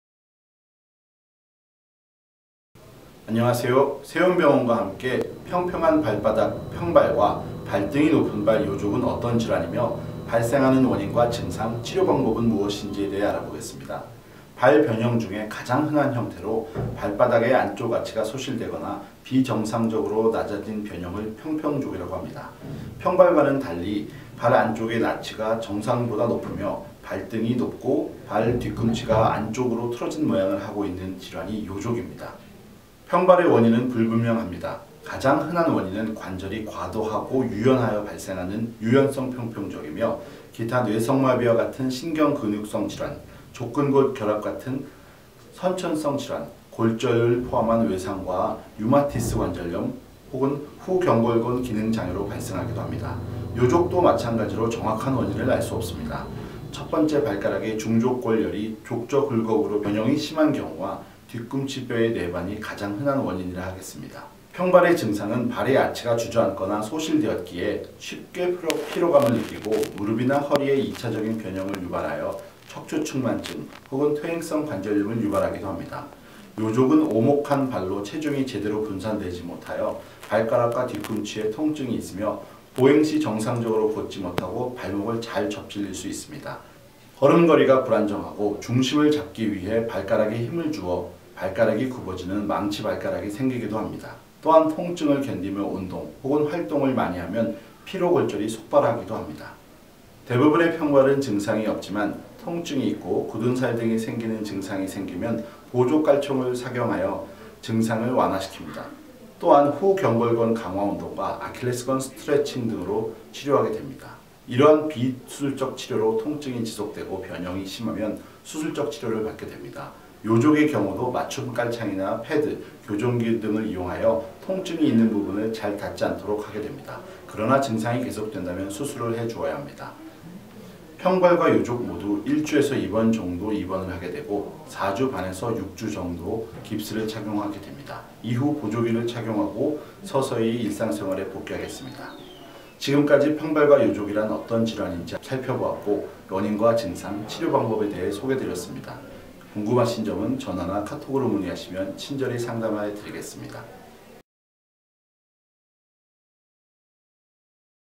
[족부질환] 평발, 요족 원인, 증상, 치료 - 이두연 원장
평발, 요족 족부질환
새움병원 이두연 원장

▶ 새움병원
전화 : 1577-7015